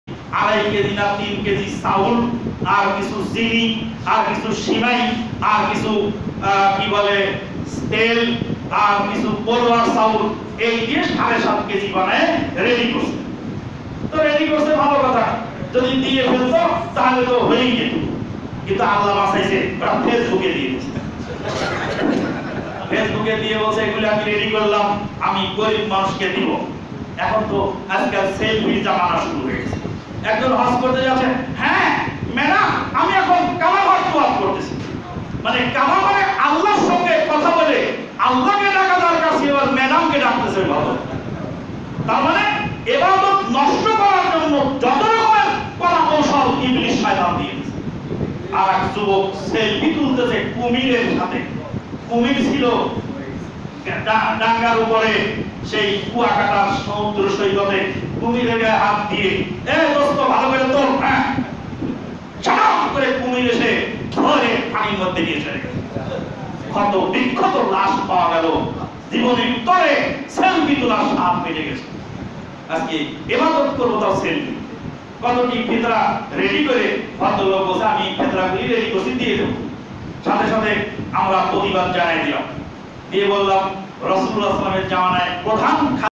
আল্লাহকে ডাকার সময় ম্যাডামকে ডাকছে! এই ছেলফির কারণে।
শায়খ আমানুল্লাহ বিন ইসমাঈল মাদানী।
Amanullha bin ismail madane